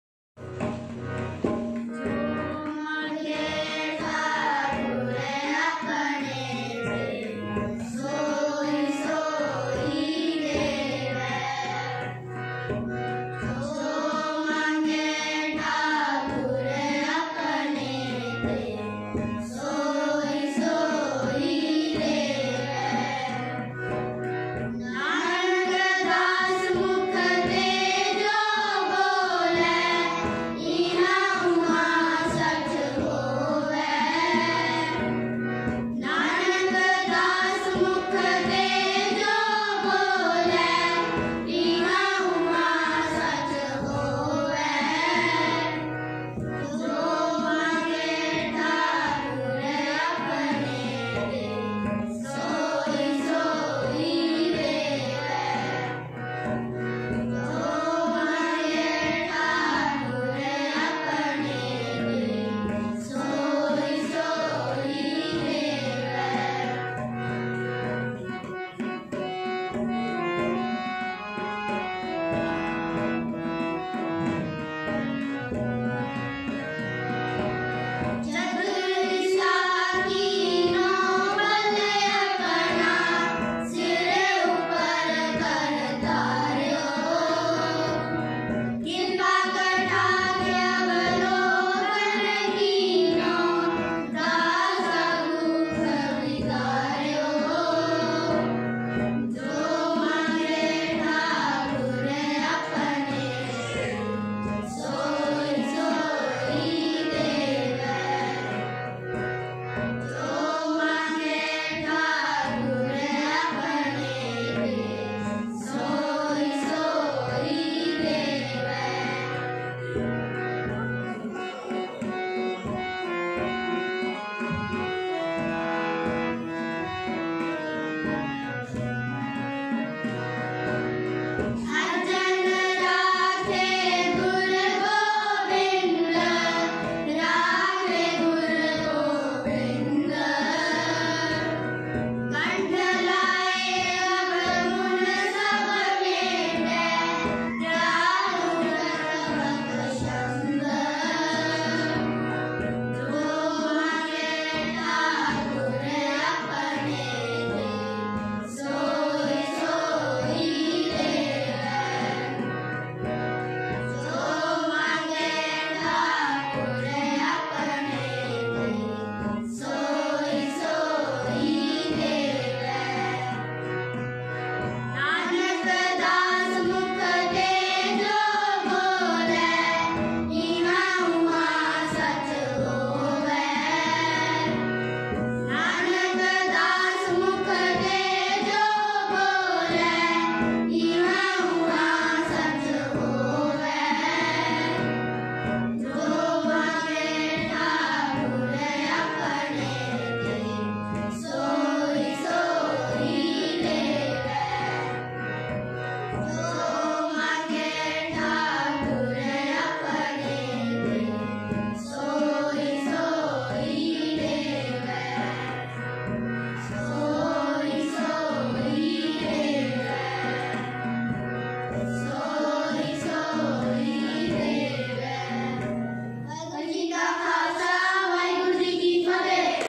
Children's Day Celebration in Victoria International School,Tanda.(4)
Participated by students from Grade Nry to 2nd .